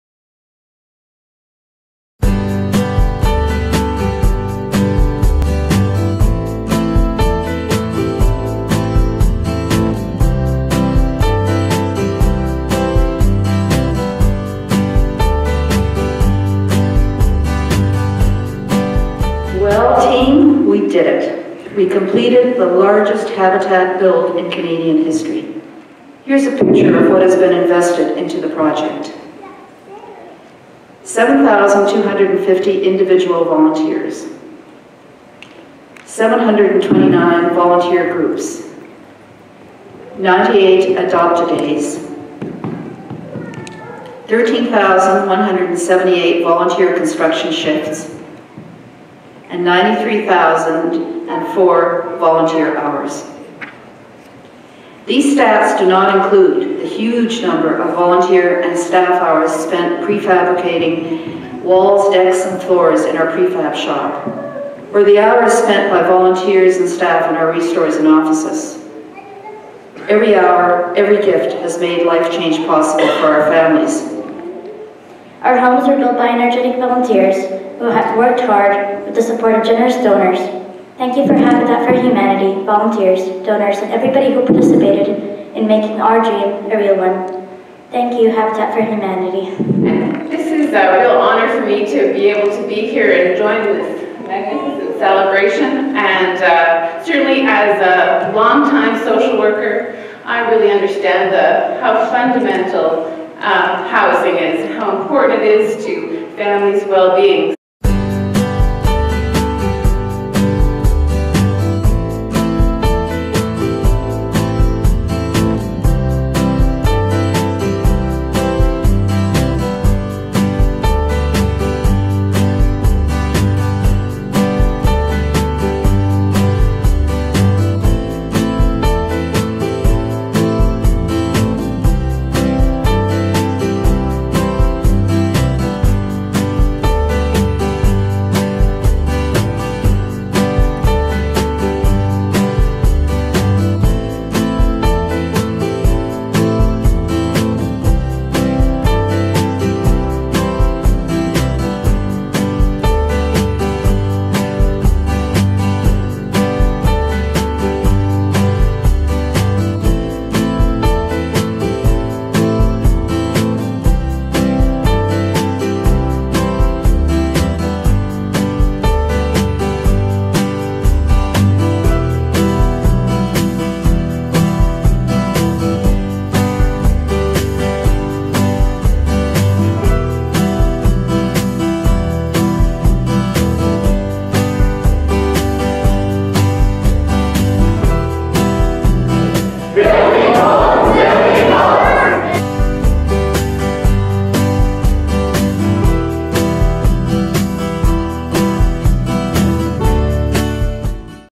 Neufeld Landing Final Home Dedication Ceremony
On December 15, 2016, Habitat for Humanity Edmonton celebrated the completion of the largest Habitat build in Canadian history (to date). Sixty-four families now enjoy home ownership because of the hard work of donors, volunteers, and staff at Neufeld Landing. The development was named after Habitat Edmonton volunteer Don Neufeld, who had contributed more than 16,000 hours since he first began volunteering with Habitat in the 1990s.